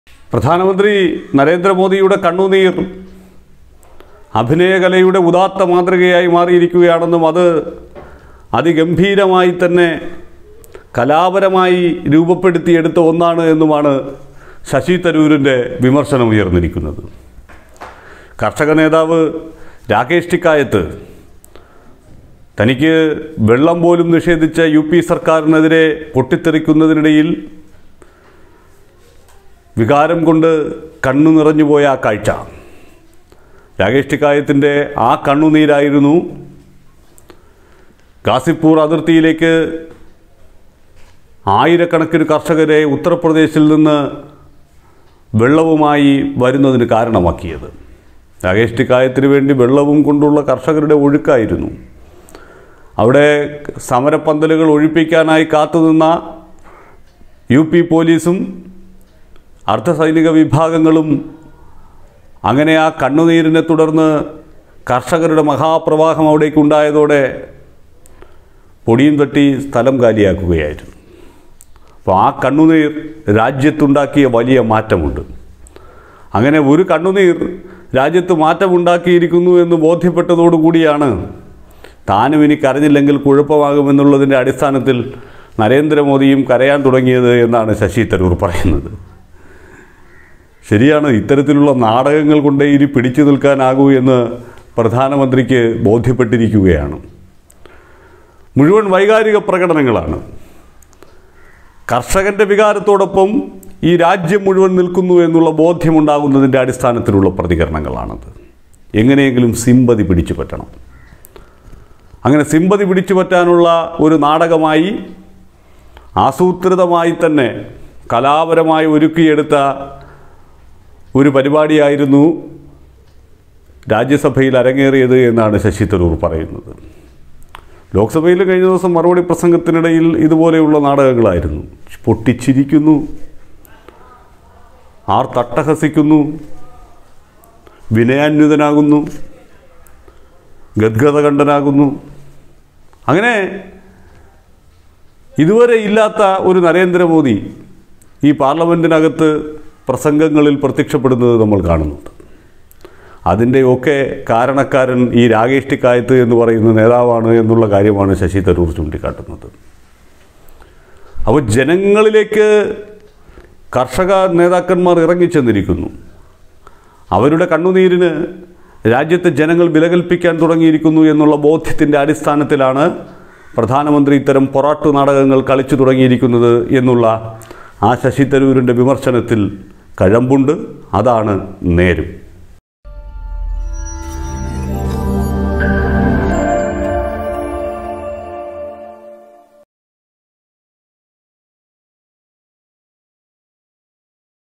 മോദിയുടേത് അനുകരണം..കുറച്ചുകൂടി മികച്ചതാക്കാമായിരുന്നു
മോദിയുടെ അഭിനയത്തിന് മാര്‍ക്കിട്ട് തരൂര്‍#